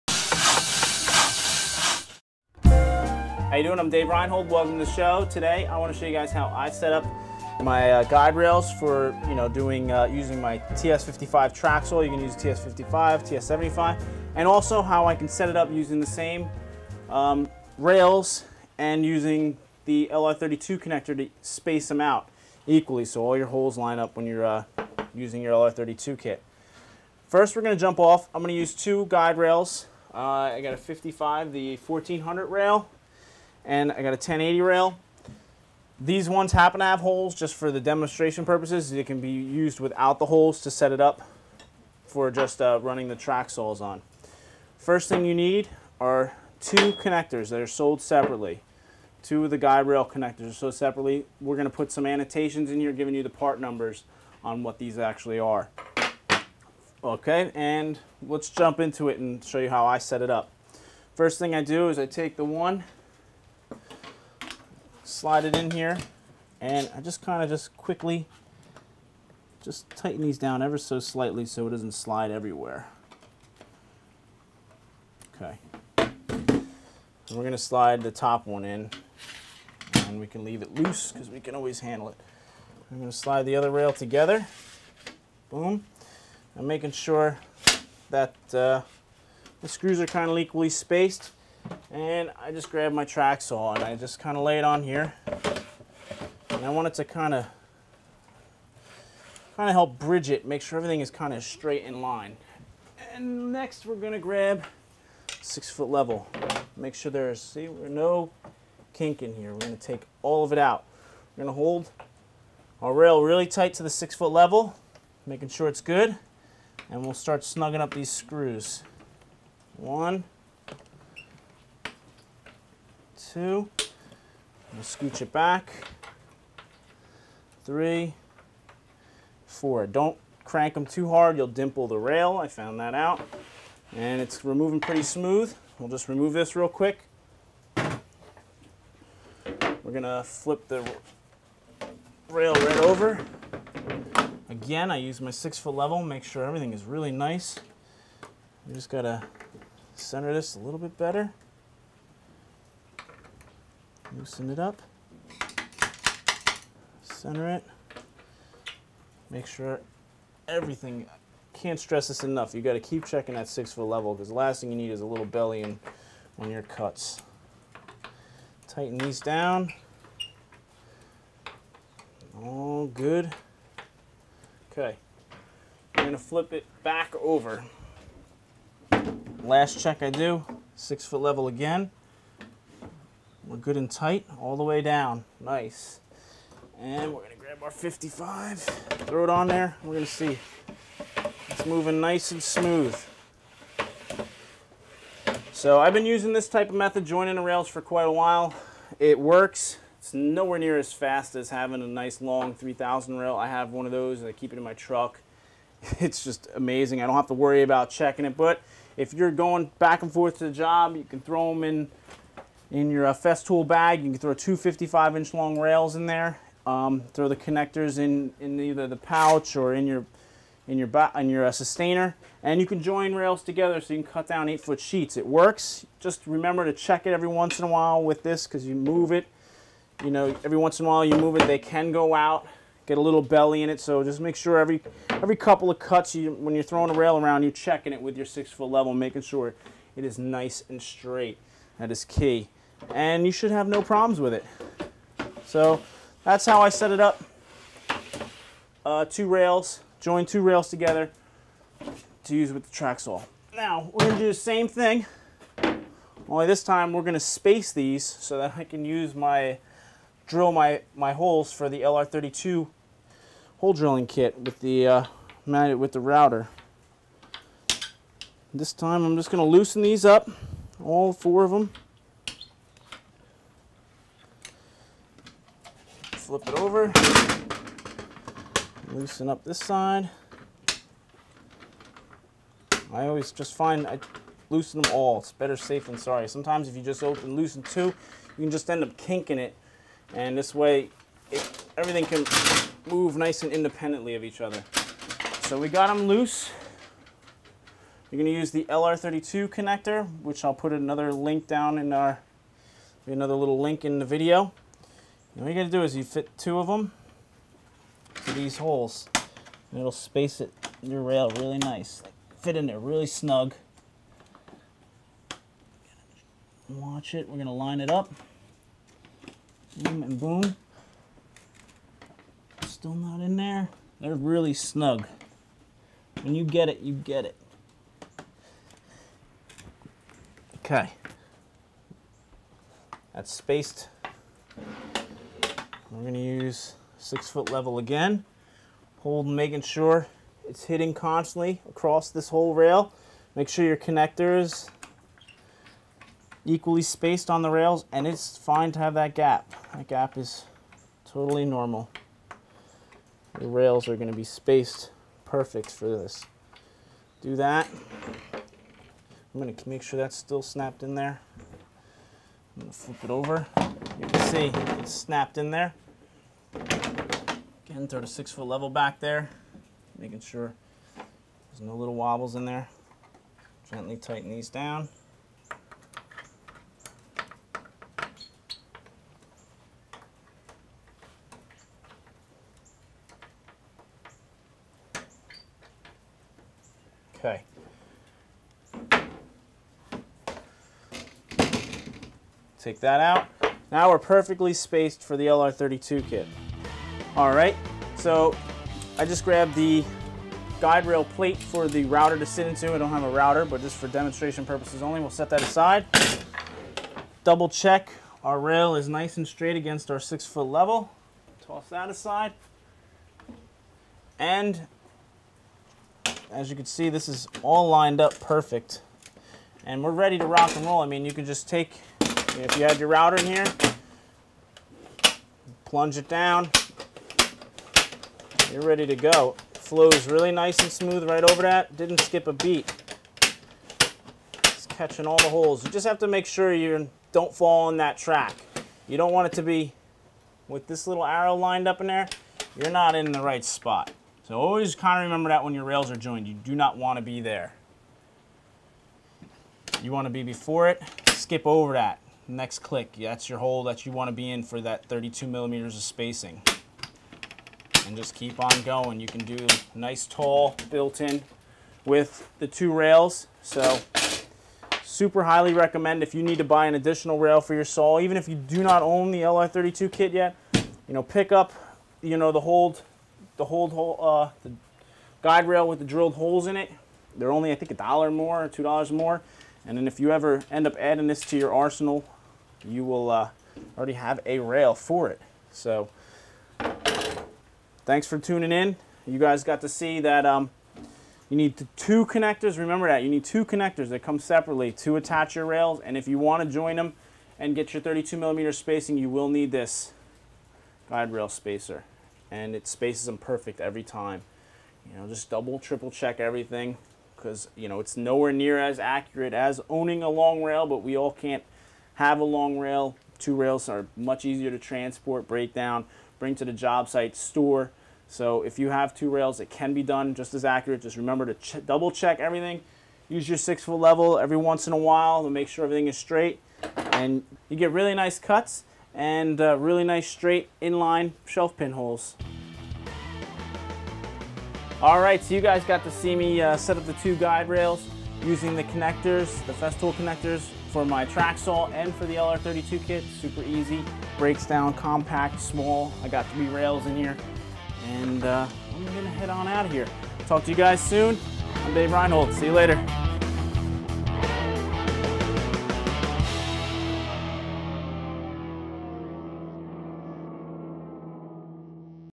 Joining The Festool Guide Rails
In this video I would like to show how I join 2 of my festool guide rails together.  I show how to set up for use with my TS-55 and also show how I space the rails with the LR32 connector.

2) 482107 Guide rail connectors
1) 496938 LR32 guide rail spacer